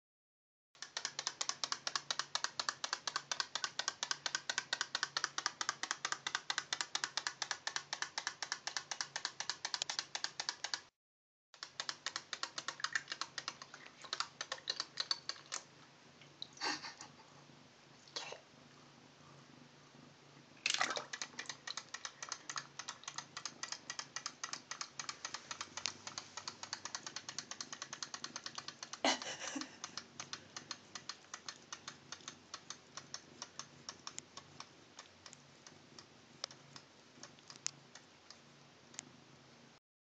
Pacer and the Toy Frog
Pacer is thirsty, but takes a moment to stop that annoying frog!